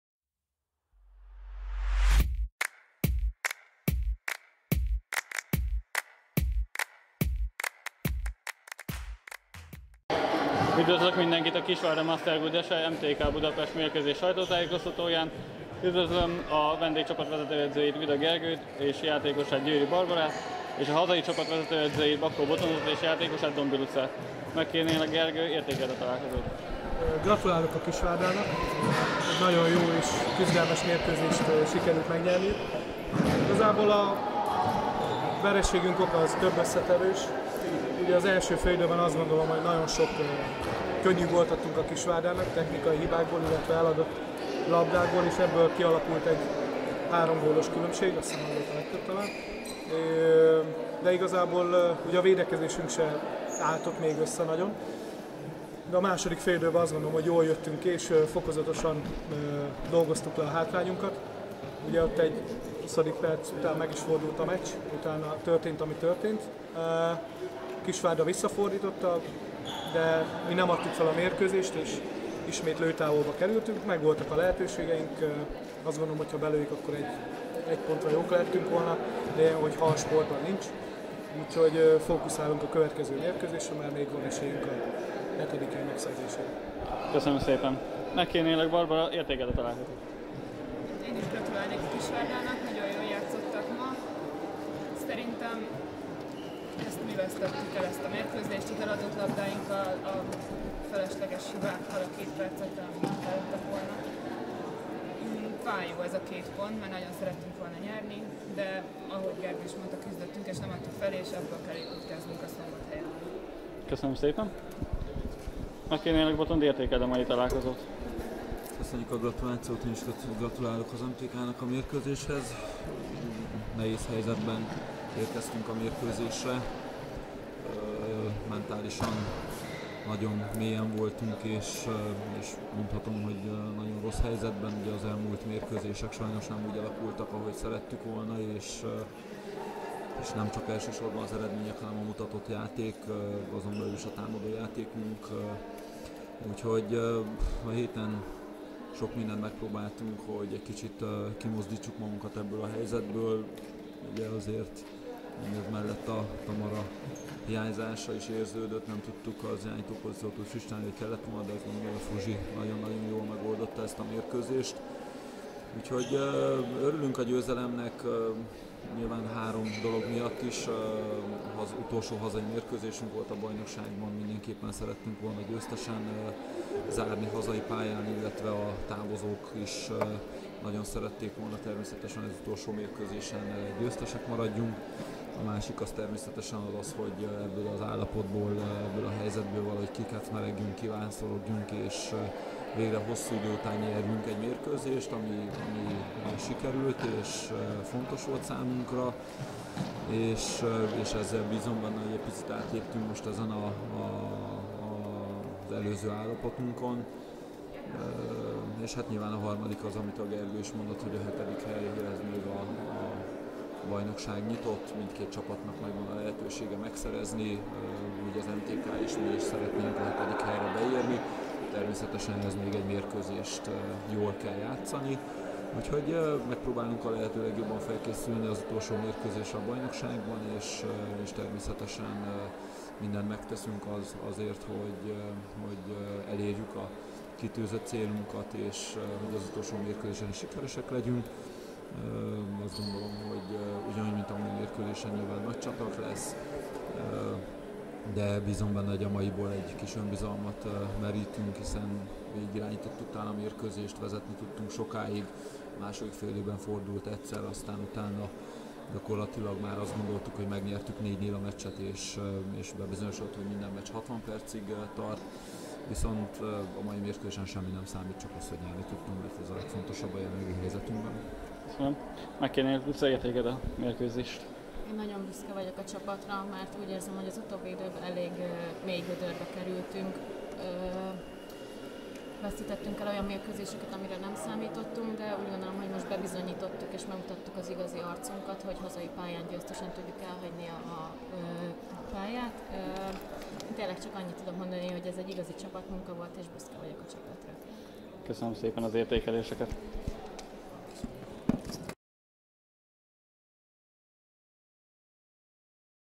Kisvárda Master Good SE - MTK Budapest Sajtótájékoztató
📣 Végeredmény: 28 - 27
🏆 K&H LIGA XXV. forduló

Vendég csapat:
▶ Vida Gergő
▶ Győri Barbara
Hazai csapat:
▶ Bakó Botond
▶ Dombi Luca

Mobil alkalmazás ► Kisvárdai Kézilabda Club